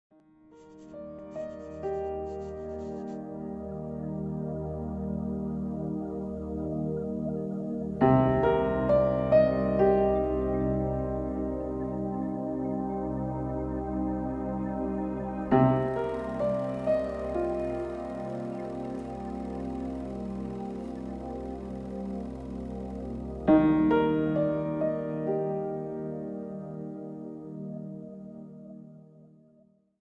Etna Silvestri Crater @ 2000m c
Mount Etna Silvestri Crater at 2000m on 12 March 2023. c.